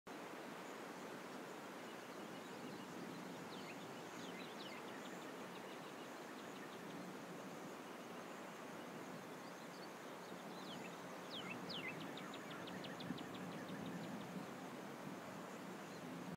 【4K】Guerrilla 1「 ロイヤリティフリー NoCopyright 」ゴリラ
This is a compilation of animals’ free stock footage that you can use for your videos.